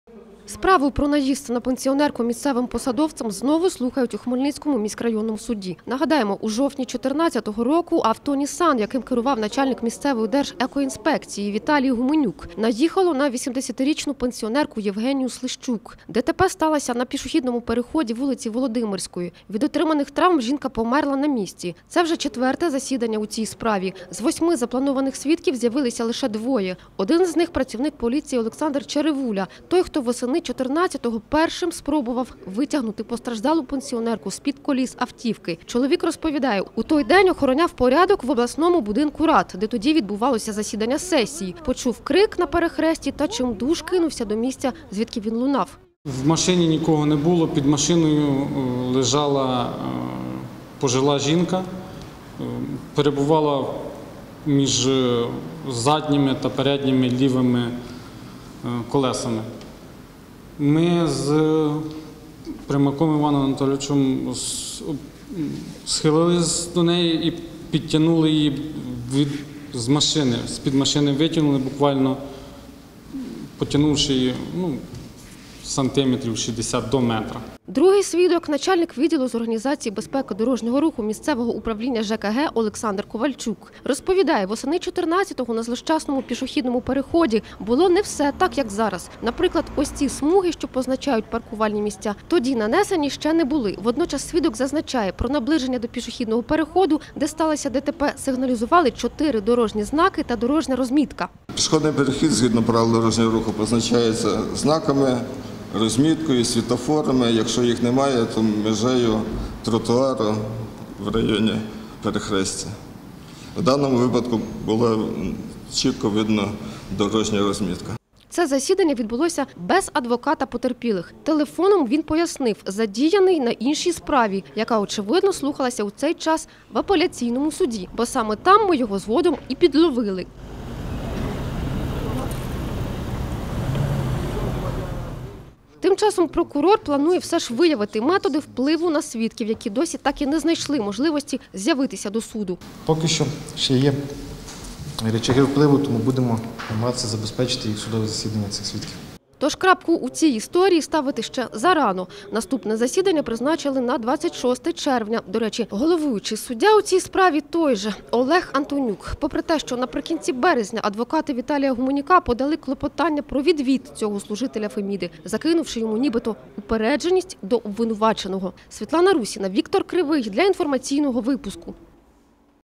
Справа начальника Держекоінспекції Віталія Гуменюка: хто саботує судові засідання?
У Хмельницькому міськрайонному суді відбулося четверте засідання у справі загибелі  80літньої жінки під колесами автівки  місцевого високопосадовця.  Йдеться про ДТП майже трирічної давнини ,  що сталося у центрі Хмельницького за участі  начальника Держекоінспекції Віталія Гуменюка.